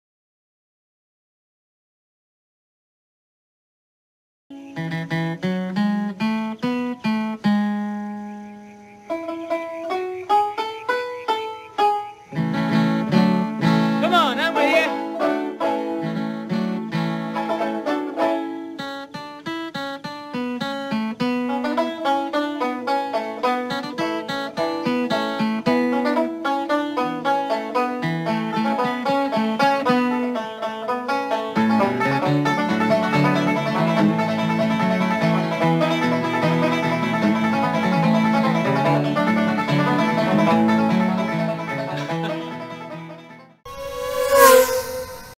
Dueling Transmitters
Setting the tension on transmitter sticks is a lot like tuning a banjo.